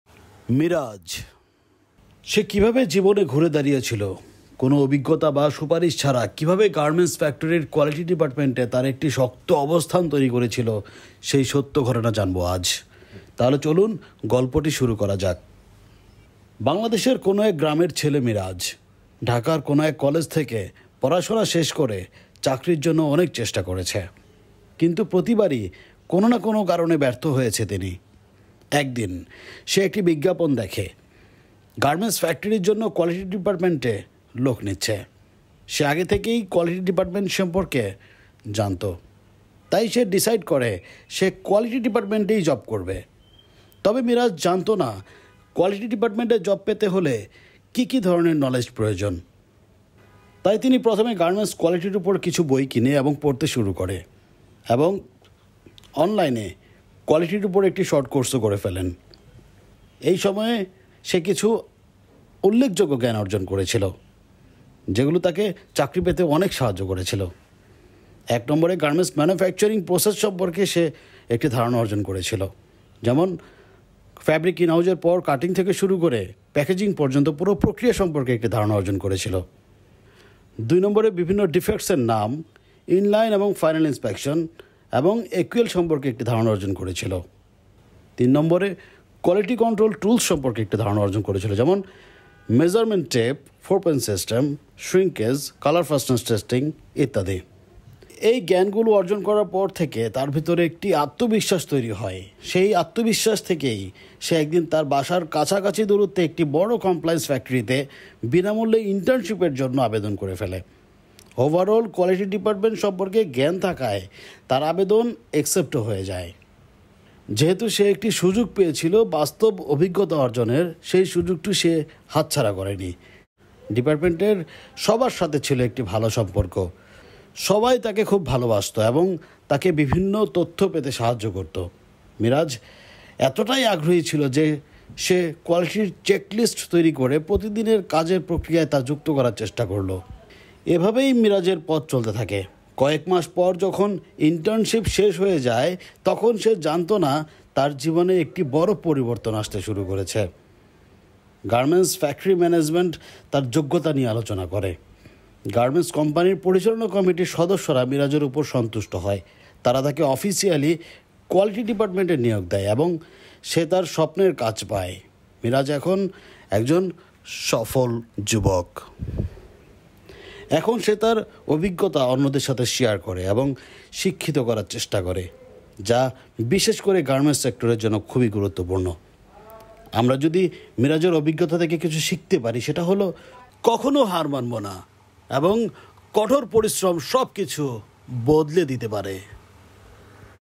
মিরাজের সাফল্যের গল্প: "এক শিক্ষিত বেকারের উত্থান" | True Story 1
মিরাজের জীবনের এই গল্পটি এক শিক্ষিত বেকারের কষ্ট, অধ্যবসায়, এবং সাফল্যের পথচলার কথা বলছে। কীভাবে কোনো অভিজ্ঞতা ছাড়াই তিনি গার্মেন্টস ফ্যাক্টরির কোয়ালিটি ডিপার্টমেন্টে নিজের অবস্থান শক্ত করেছেন, সেটাই তুলে ধরা হয়েছে এখানে। মিরাজের সংগ্রাম, জ্ঞান অর্জনের প্রচেষ্টা, এবং বিনামূল্যে ইন্টার্নশিপের মাধ্যমে নিজেকে তৈরি করার গল্পটি আপনাকে অনুপ্রাণিত করবে। এই গল্প থেকে আপনি শিখবেন যে কঠোর পরিশ্রম ও আত্মবিশ্বাসের মাধ্যমে জীবনের কঠিন সময়কে কীভাবে জয় করা যায়।

আপনার নিজের অভিজ্ঞতা আমাদের সাথে শেয়ার করতে ভুলবেন না!